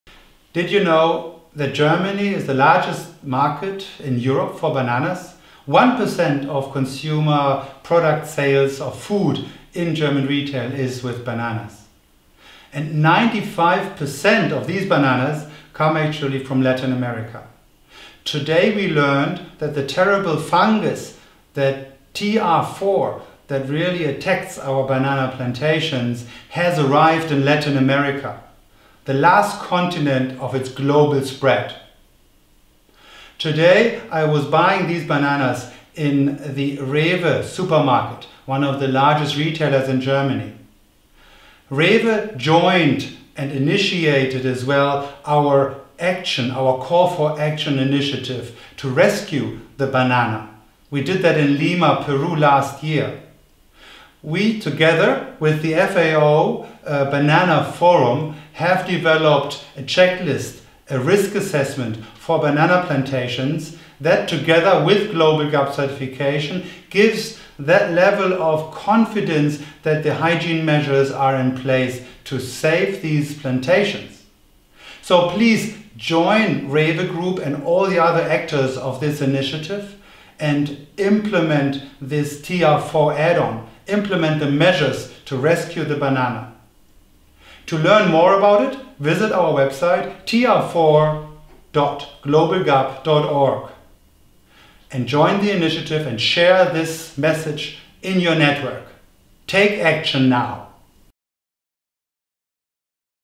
Call to action to save the banana
TR4 has now arrived in Colombia. The GLOBALG.A.P. Secretariat has developed the TR4 Biosecurity Add-on, a tool that provides farmers with a risk mitigation plan to prevent the introduction (and/or further spreading) of the TR4 pathogen